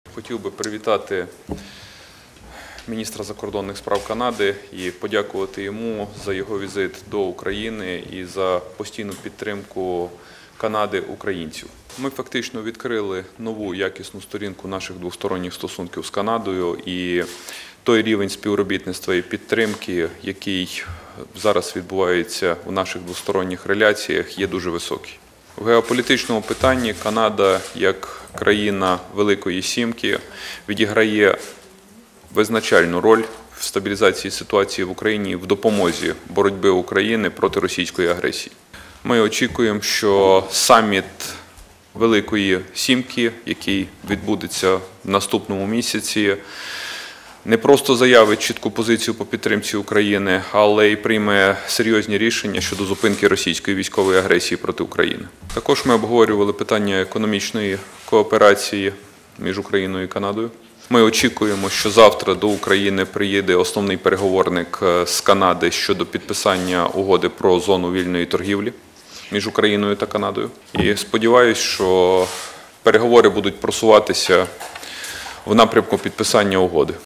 «Очікуємо, що саміт Великої сімки прийме серйозні рішення щодо зупинки російської військової агресії
«Ми очікуємо, що саміт Великої сімки, який відбудеться наступного місяця, не просто заявить чітку позицію з підтримки України, а й прийме серйозні рішення щодо зупинки російської військової агресії проти України», - наголосив Прем’єр-міністр України, лідер партії «Народний фронт» Арсеній Яценюк у вівторок, 12 травня, по завершенні зустрічі з Міністром закордонних справ Канади Робертом Ніколсоном.
Він підкреслив, що Канада як країна Великої сімки відіграє визначальну роль в стабілізації ситуації в Україні і в допомозі в боротьбі України проти російської агресії.
Глава Уряду України повідомив, що під час зустрічі з Робертом Ніколсоном були обговорені питання економічної кооперації між Україною і Канадою.
За словами Арсенія Яценюка, Уряд очікує, що 13 травня до України прибуде «основний переговорник з Канади щодо підписання угоди про зону вільної торгівлі між Україною та Канадою»: «Сподіваюся, що переговори будуть просуватися в напрямку підписання угоди».
Від імені Уряду України Арсеній Яценюк висловив вдячність канадському Уряду і персонально Прем'єр-міністру Канади Стівену Харперу «за високу підтримку України, українців і нашої спільної боротьби за вільний світ».
«Ми фактично відкрили нову якісну сторінку наших двосторонніх відносин із Канадою. І той рівень співробітництва і підтримки, який зараз відбувається в наших двосторонніх реляціях, є дуже високий», - підкреслив Прем’єр-міністр.

Приєднуйся!